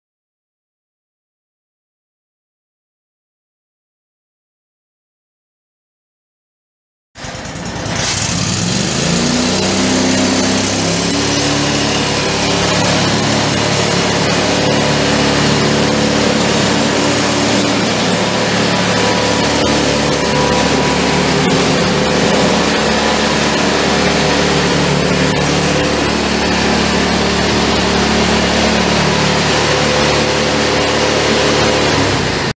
Dek Brush Beast brush mower for sale | no-reserve Internet auction December 27, 2017
View a detailed listing of this item and place bids

Dek Brush Beast brush mower
84 hours on meter
36" cut
Subaru gas engine
Electric start
Serial 36BB1000064
It's located in Newton, KS, and we're selling it to the highest bidder regardless of price.